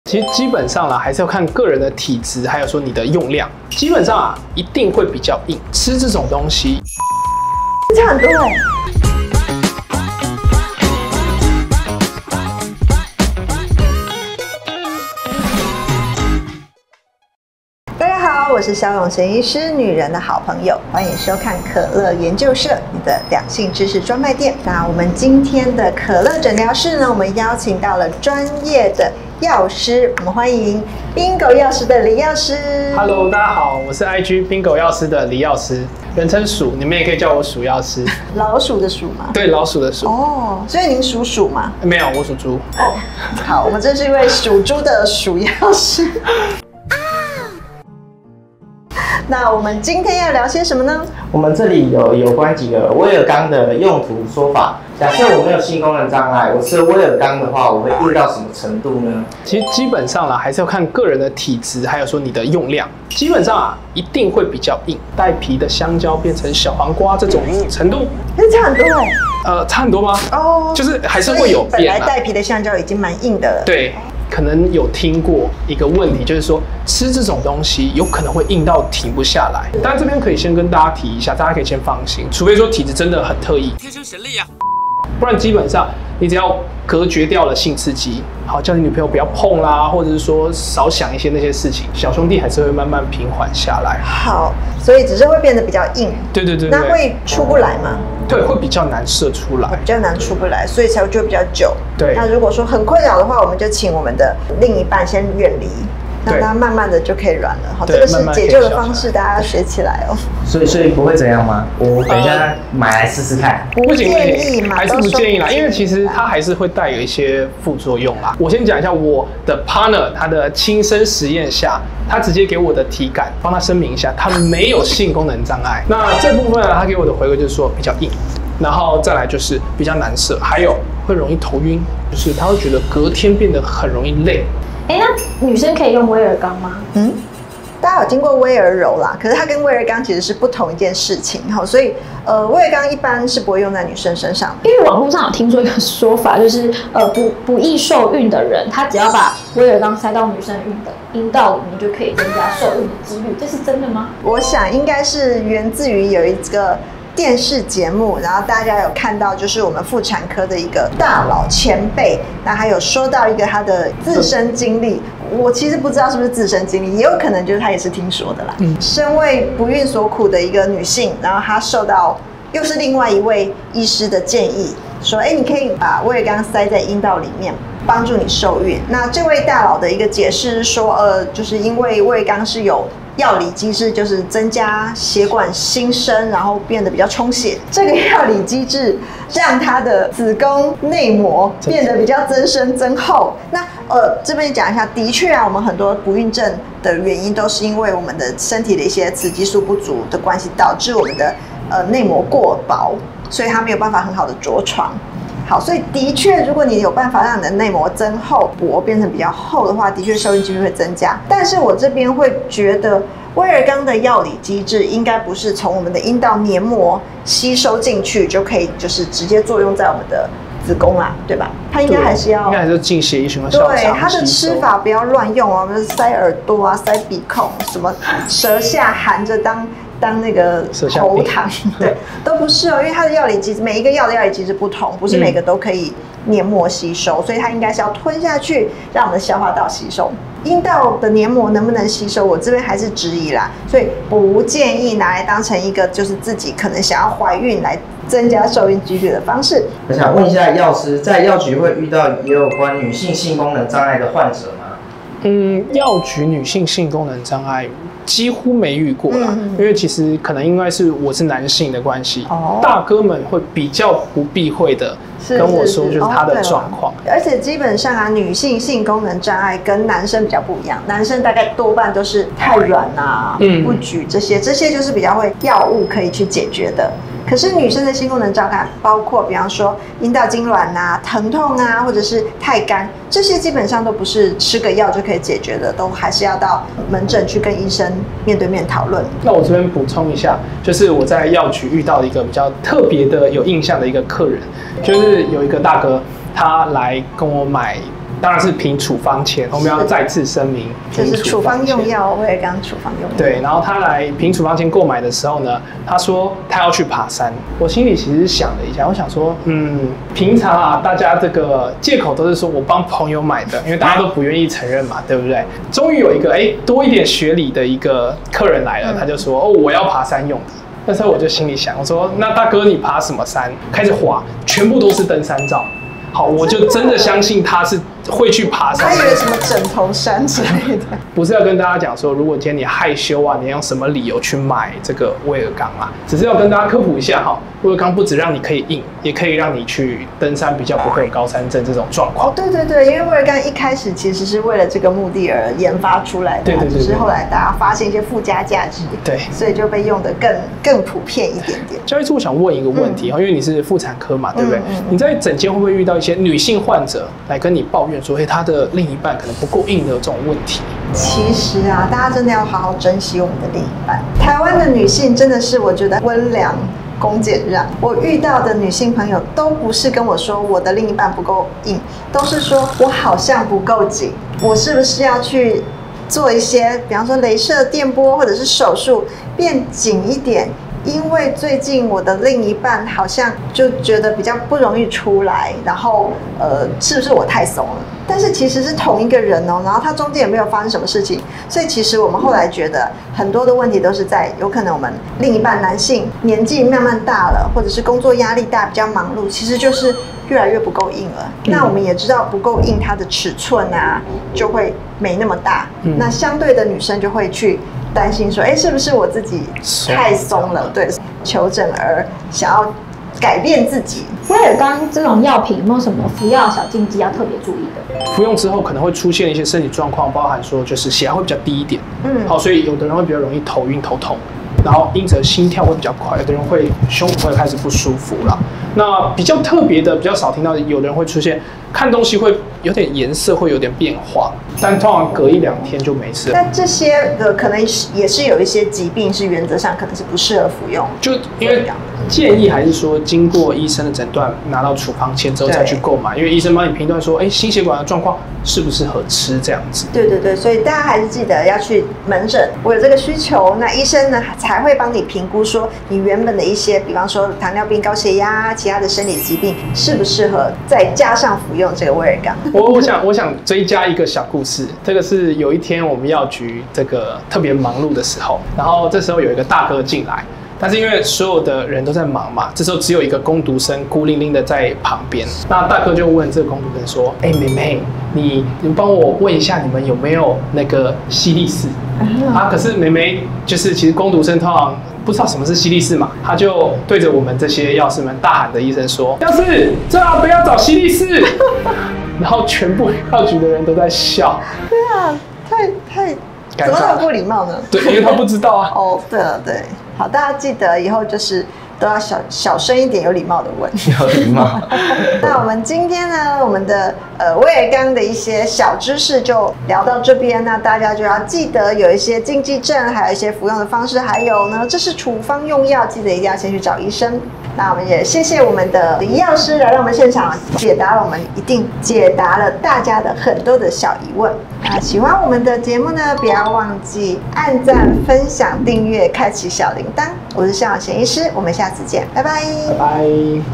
軟男救星！一吃就硬！Ft黎藥師｜可樂診療室｜女人的好朋友｜蕭詠嫻醫師
俗稱『藍色小藥丸』的威爾剛
是許多男性們們的救星
但如果性功能正常的硬漢吃威爾鋼
是不是會變得更加勇猛呢？

威爾鋼除了改善性功能障礙
是否還能有其他的用途
而假設女生吃威爾鋼的話
又會有什麼效果呢？

今天的可樂診療室
邀請冰狗藥師-黎藥師
和蕭詠嫻醫師一起告訴你

重點章節
0:00 精采預告
0:30 來賓介紹
0:55 性功能正常吃威爾剛會多硬？
2:00 藥師夥伴實驗後心得
2:15 女生可用威爾剛嗎？
2:25 在女生因道塞威爾剛可幫助受孕？
4:10 性功能障礙的女性患者多？
4:52 藥師分享特殊經驗
5:54 醫師曾遇過女性患者抱怨另一半不夠硬？
6:56 威爾剛服藥禁忌

還想看什麼內容，留言告訴我們喔！
更多精彩影片清單：
觀看可樂研究社其他社群
合作資訊與洽談請洽